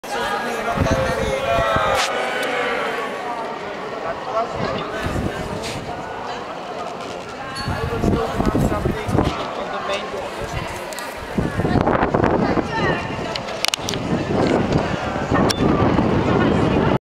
Prague Castle -- Prague, Czech Republic
A visit to the Prague Castle to see the most beautiful High Gothic architecture in all of Europe!